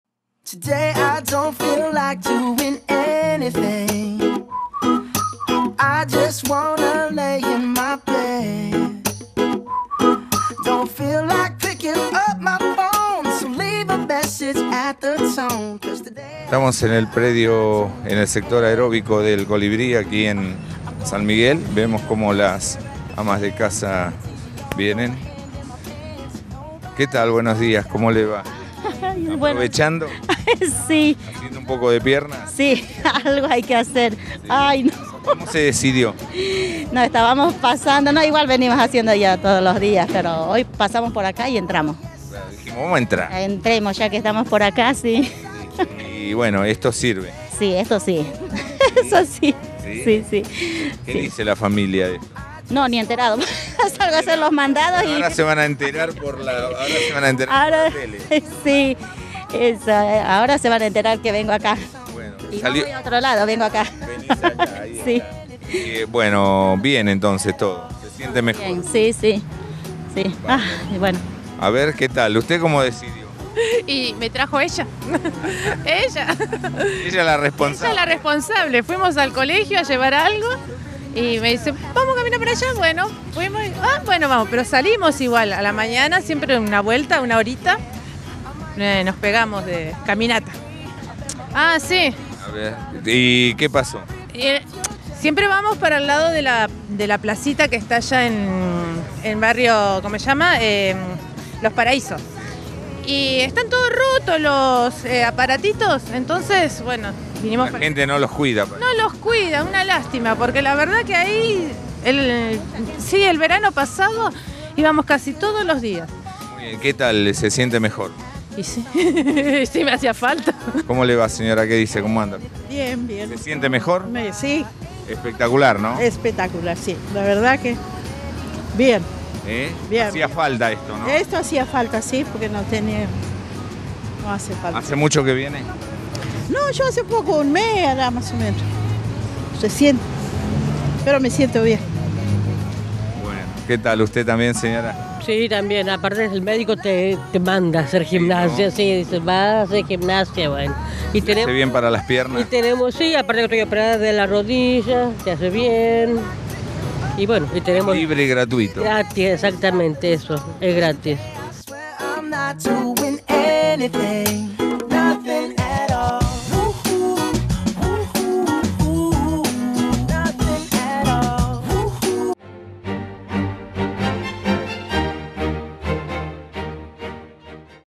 Gimnasia para amas de casa
San Miguel - Amas de casa aprovechan gratuitamente, los numerosos aparatos, para realizar todo tipo de gimnasia, en el predio Deportivo “El Colibrí”